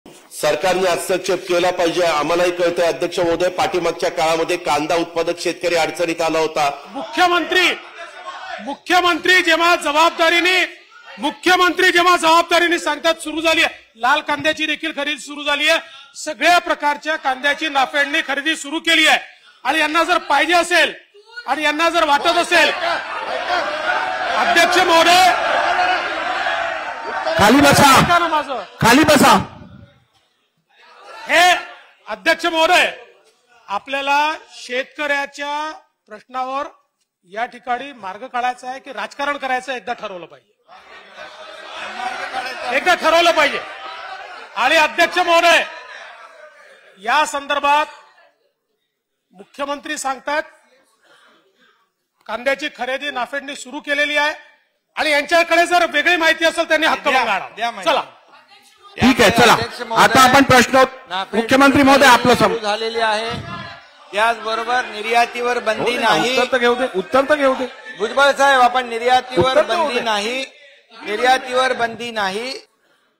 कांद्याची खरेदी नाफेडने सुरु केलेली आहे, निर्यातीवर बंदी नाही | DCM Devendra Fadnavis
(विधानसभा । दि. 28 फेब्रुवारी 2023)

सगळ्या प्रकारच्या कांद्याची नाफेडने खरेदी सुरु केली असून, विधिमंडळात शेतकर्‍यांच्या प्रश्नावर राजकारण नाही तर मार्ग काढायचा आहे.